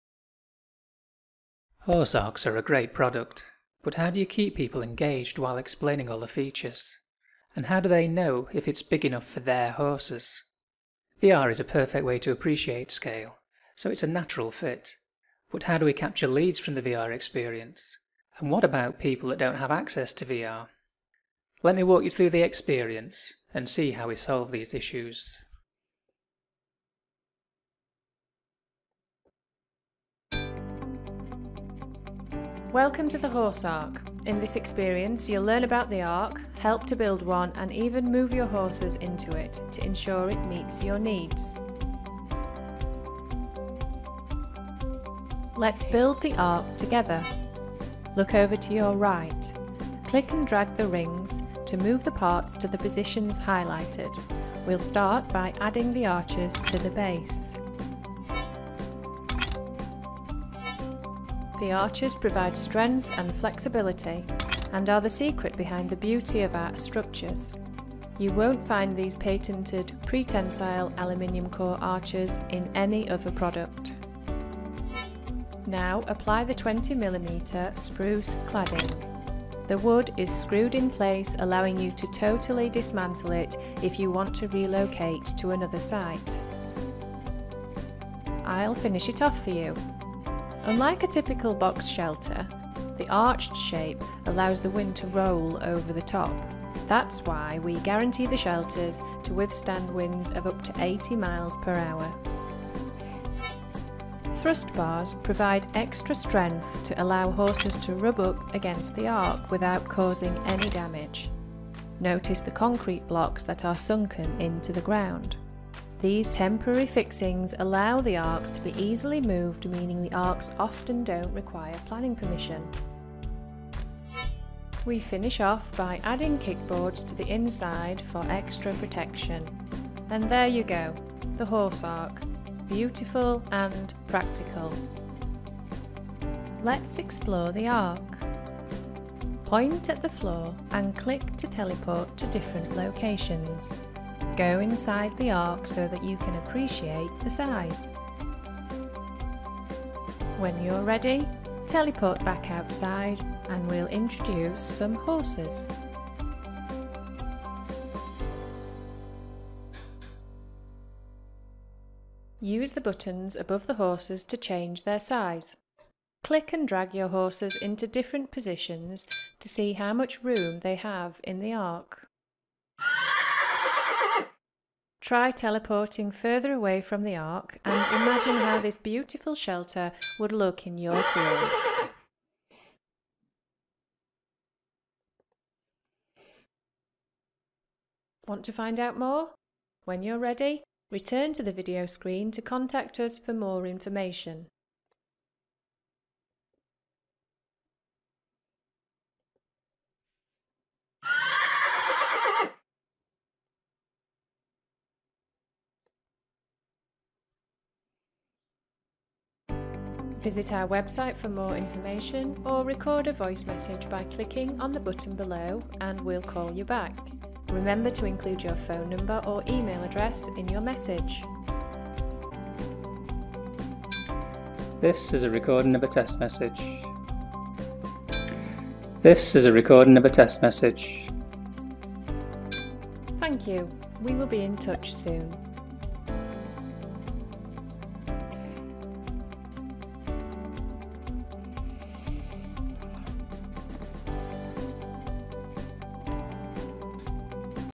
Horse Arc Product Evaluation Demo (webVR)
This webVR project was created using Amazon Sumerian and AWS services. It was created for the 2019 Amazon Sumerian AR/VR Challenge and won the "Best Brand Engagement and Retail" category. It works on mobile VR (Oculus Go tested), Desktop VR (Oculus Rift tested) and works the same in desktop browsers (Chrome tested) in a non-VR mode.